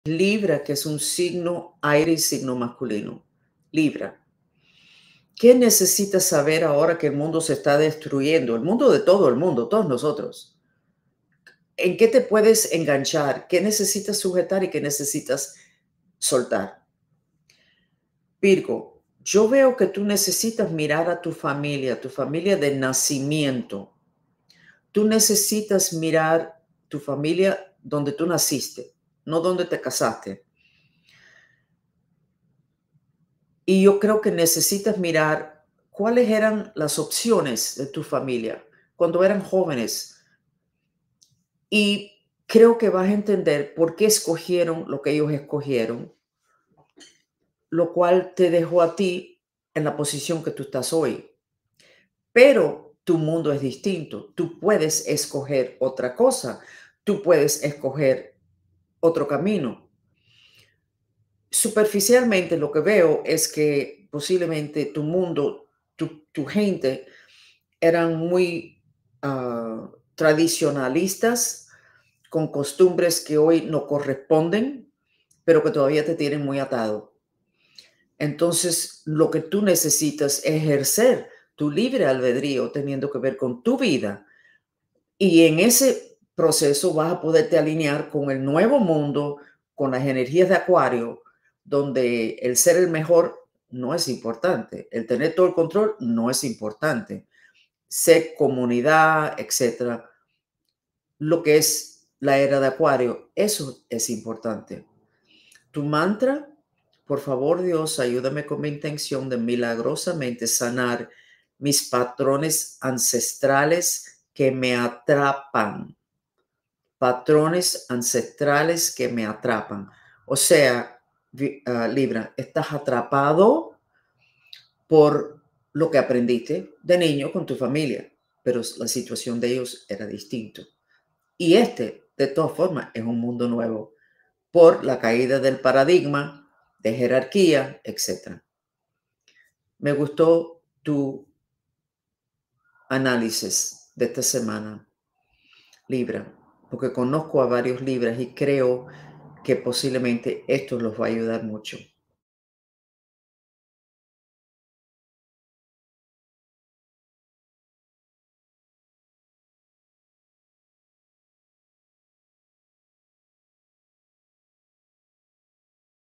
♎ Libra, Destrucción del mundo que conocíamos
Libra, Se acabó el mundo que conocíamos, como lo dijeron los Mayas.
¿Cómo mantienes tu estabilidad en medio del Caos?
Tu mantra: Por favor Dios, ayúdame con mí intención de milagrosamente sanar mis patrones ancestrales que me atrapan.

🔉  PODCASTS FRANCESFOX NOTICIAS DE OTRAS DIMENSIONES